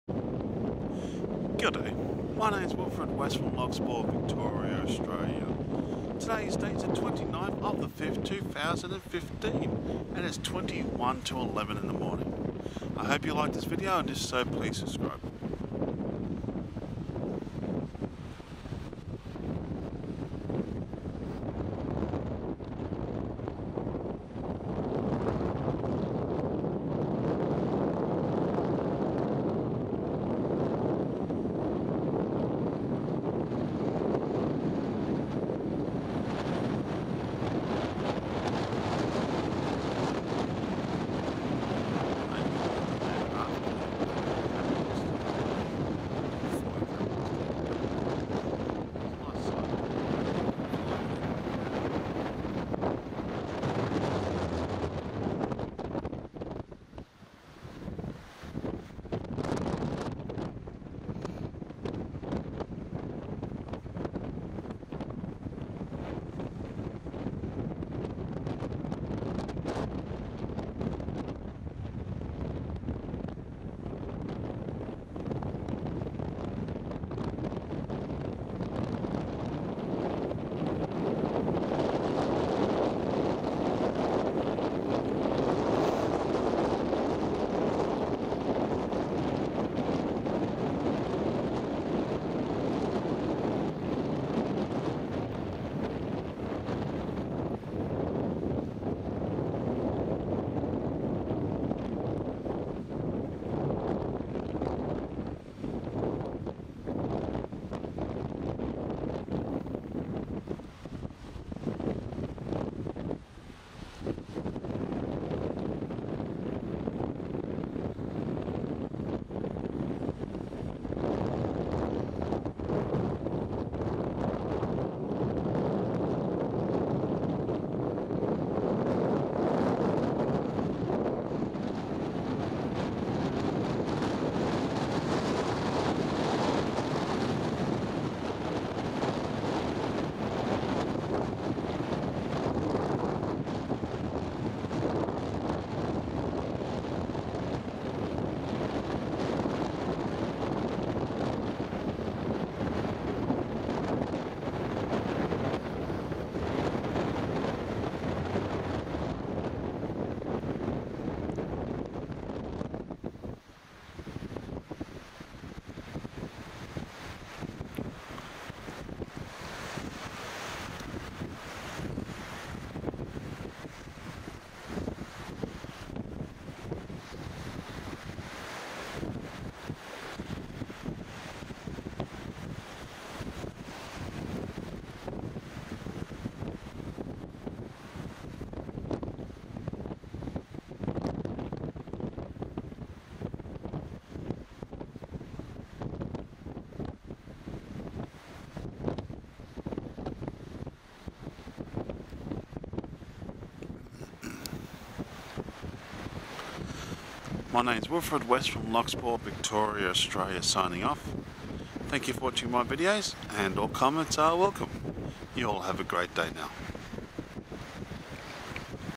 Looking at Lake Victoria on the 29-05-2015
This video was made with the Sony Video Camera on the Weifeng Tripod on the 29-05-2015 Res at 1920 x 1080 25 Fps and the voice over was made using the Sony On-board Mic and the editing was done with Adobe Premiere Pro CC, and then Exported using Abode Premiere Pro CC with the res set at 1920 x 1080 with 25 Fps and the Size is 216 MB Length is 3:38 Video Type MP 4 and uploaded to YouTube with the res at 1920 x 1080 with 25 Fps.